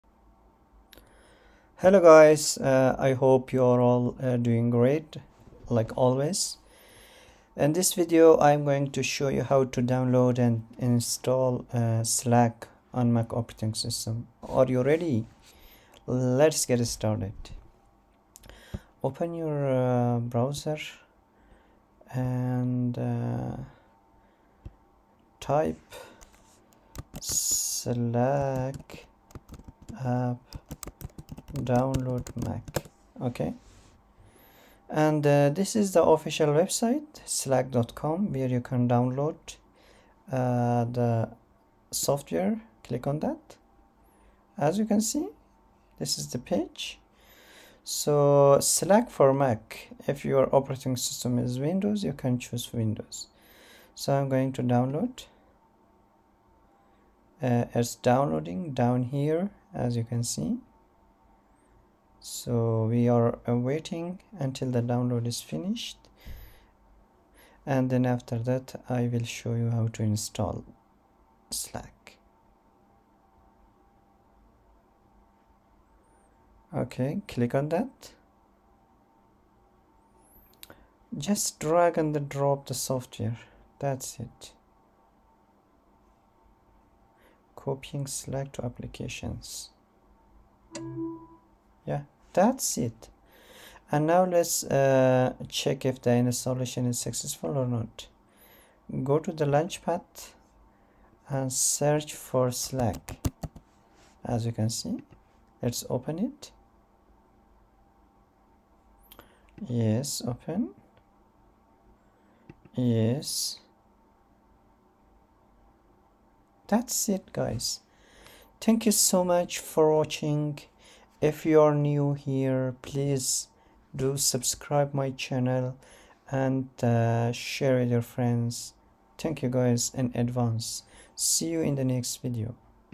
How to download and install Slack on Mac OS
Hi Friends
In this video I will show you how to download and install slack on mac os for free.